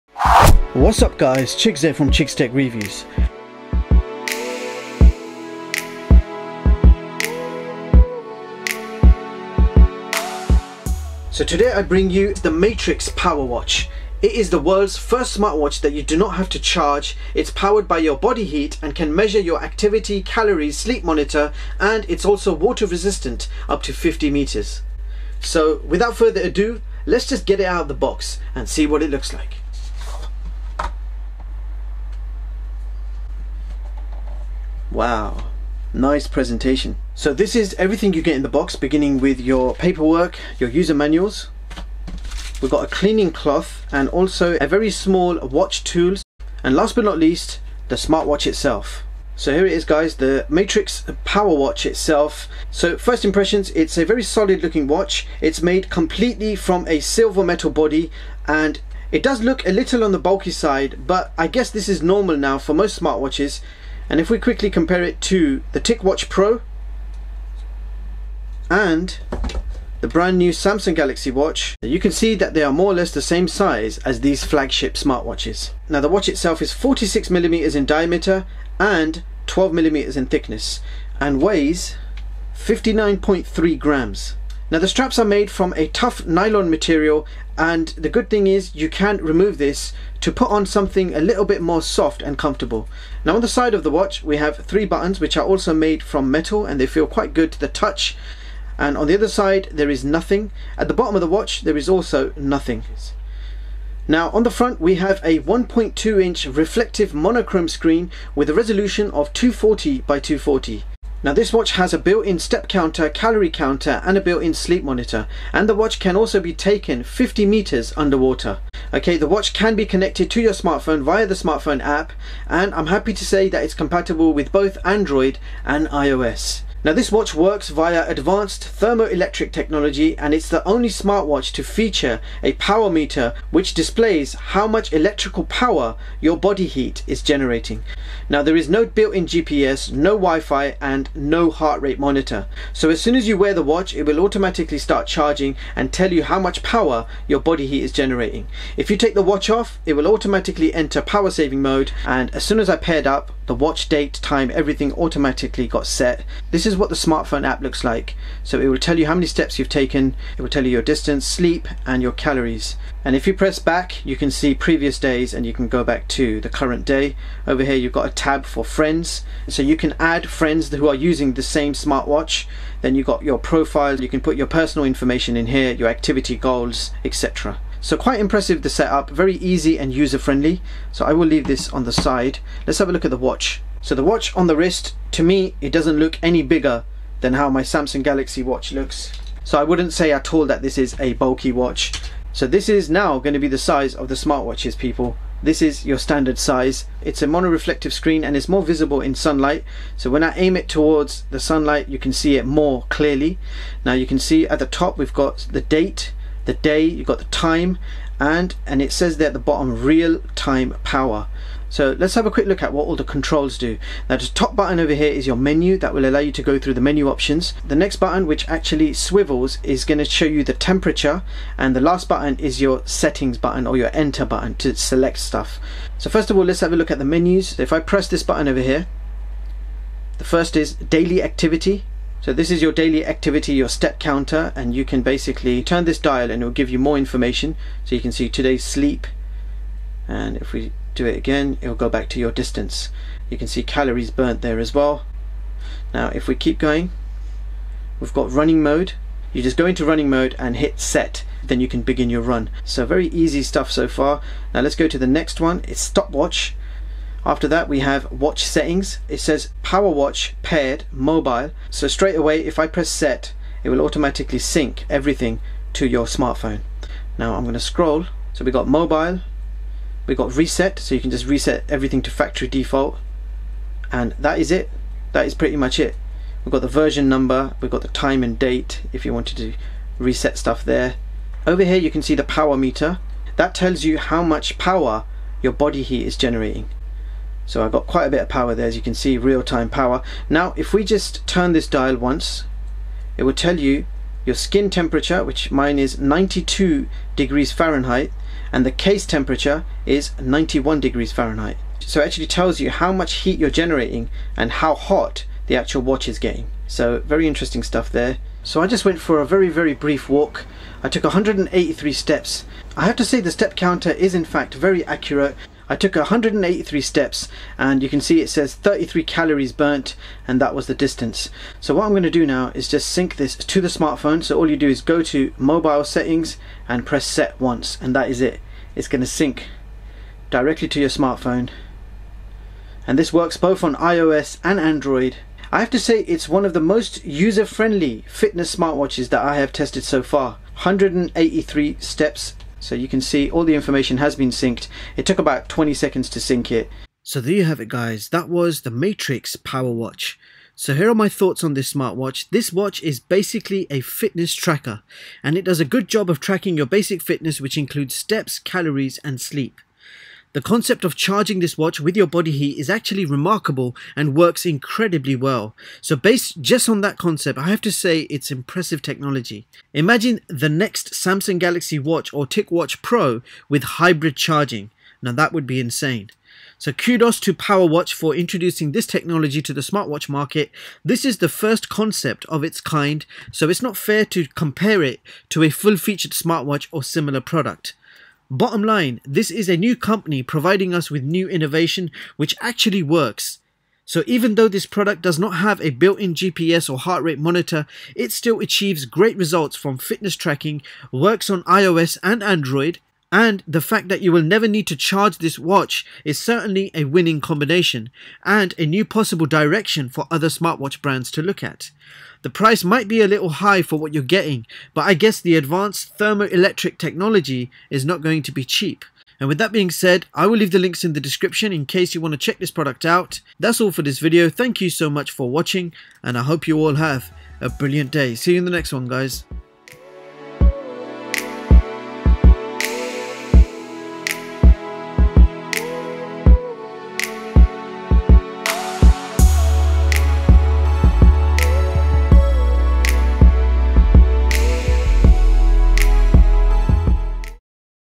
MATRIX POWERWATCH - Fitness Smartwatch Powered By Body Heat
Specs:
1.2 inch Reflective Monochrome Display
Resolution of 240x240
Built in Step Counter
Built in Calorie Counter
Built in Sleep Monitor
50m Water Resistant
Connectivity: Android/iOS
Removable Straps
46mm diameter and 12mm thick
60g
NO GPS
NO WiFi
NO Heart Rate Monitor